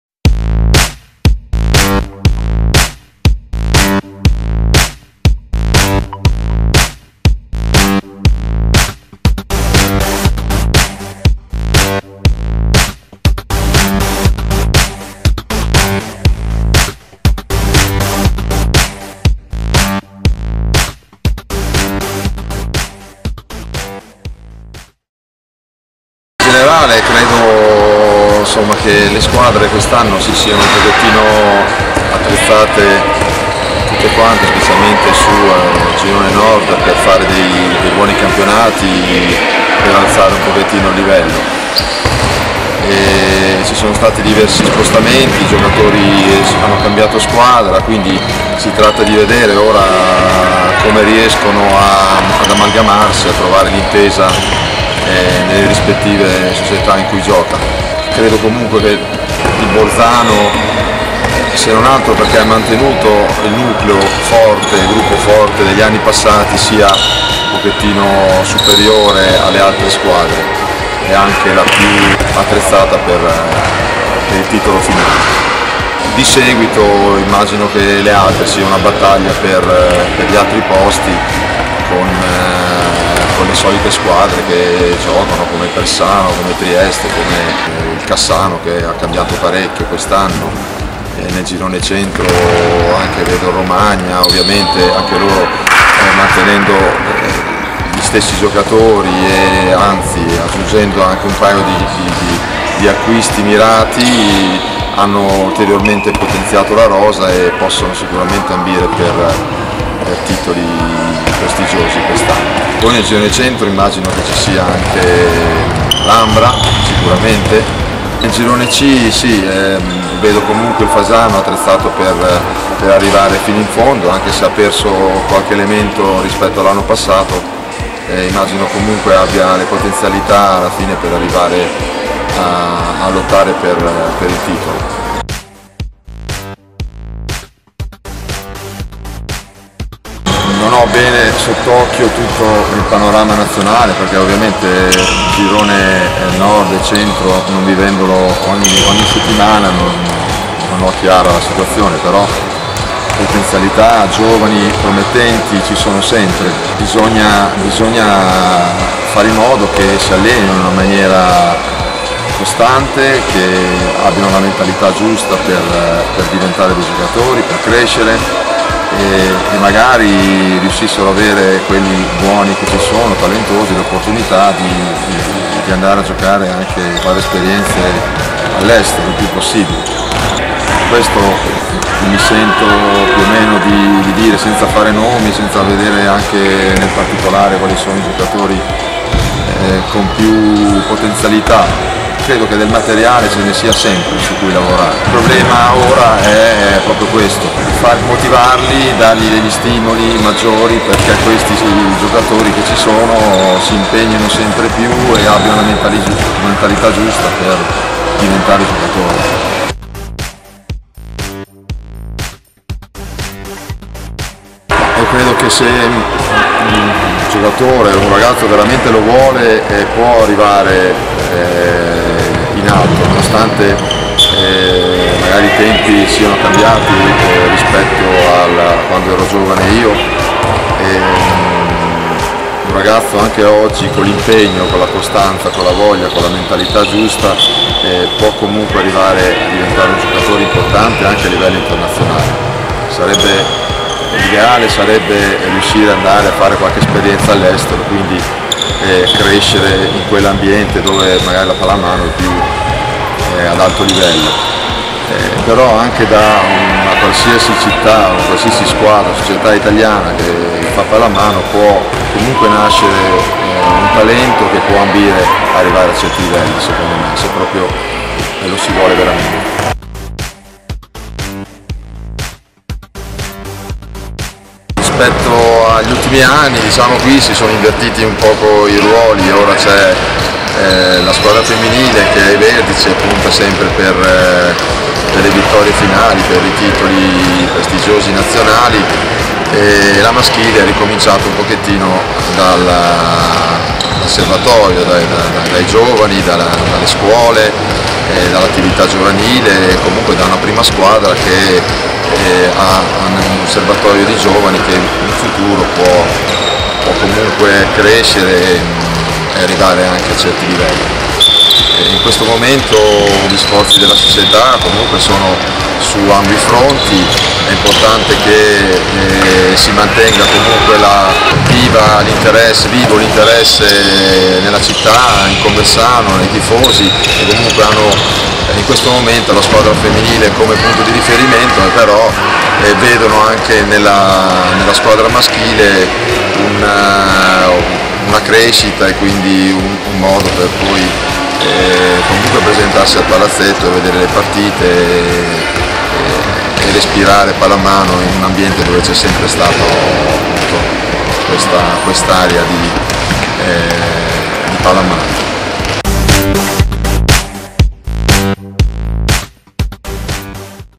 PallamanoTV con... Alessandro Tarafino
Intervista ad Alessandro Tarafino, allenatore del Conversano ed ex capitano della Nazionale maschile.